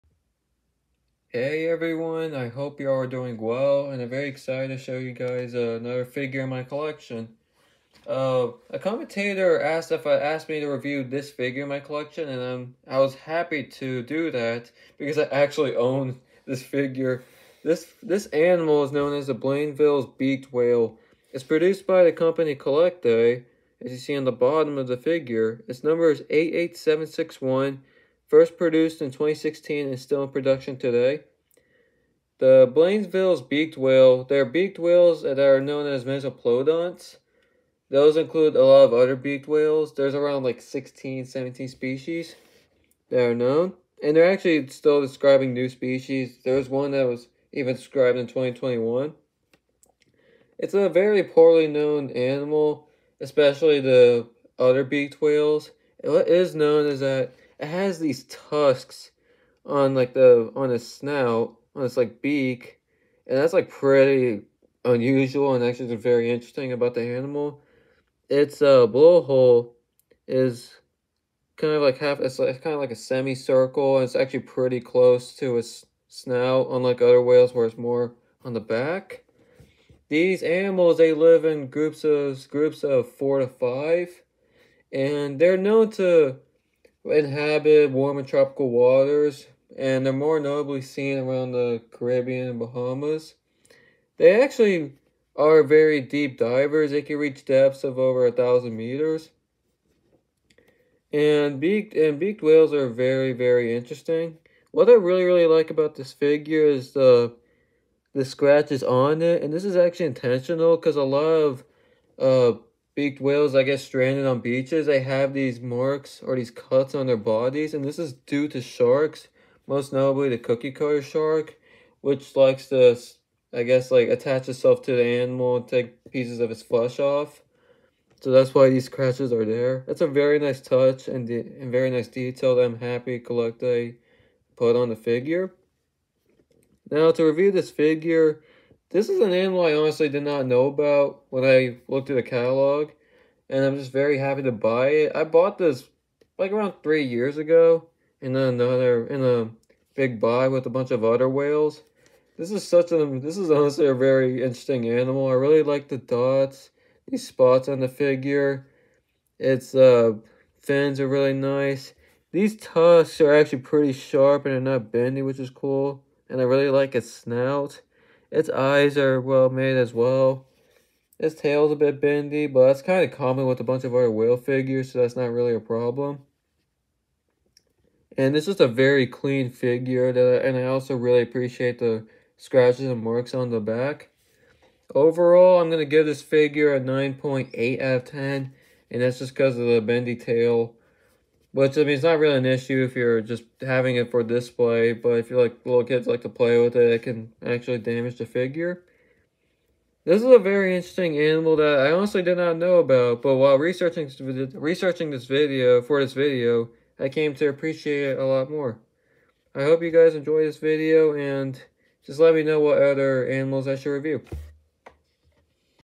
CollectA Blainville’s Beaked Whale Review
Reviewing the unusual Blainville’s beaked whale produced by CollectA 88761. The Blainville’s beaked whale, along with other beaked whales, are mesoplodonts with tusks protruding out of their beaked snouts. They have wide ranges in warm and tropical waters and are often seen with shark bites and scratches on their bodies. This is a relatively unknown but unique animal I’m happy to own.